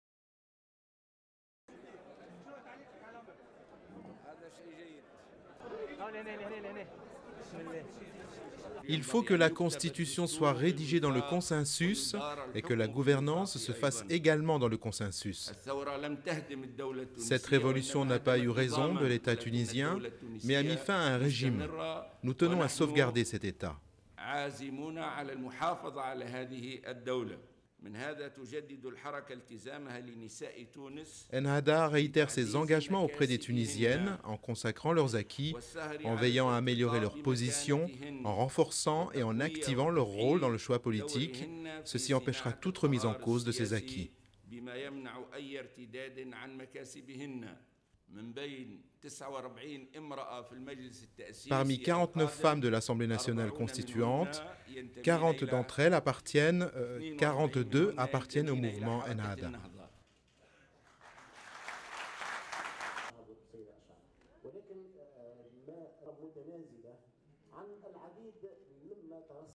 Tunisie: les islamistes s'engagent pour la démocratie
Vainqueurs des premières élections libres en Tunisie, les islamistes d'Ennahda ont insisté vendredi 28 octobre sur leur engagement démocratique, mais des violences ont éclaté dans la ville symbole de la révolution, Sidi Bouzid. Discours. Durée: 01:09.